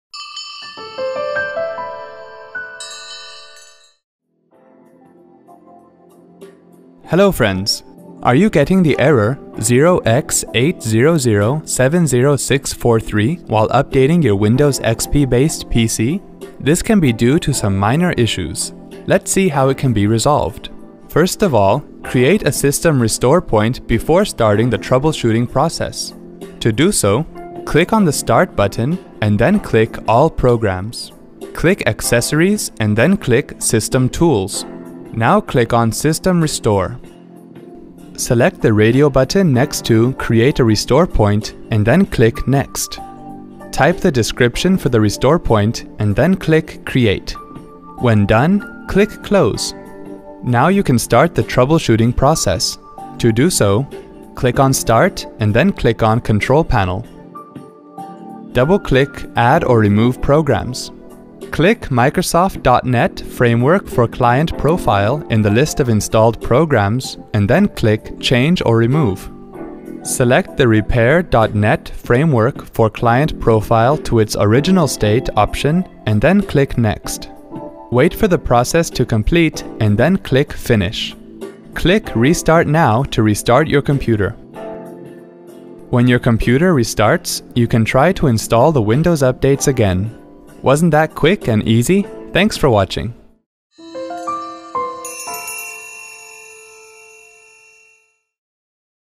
Windows® Update Error 0x80070643 in Windows® XP
If you wish to fix the "Update error code 0x80070643" in a Windows® XP-based PC , then follow the easy steps shown in this video.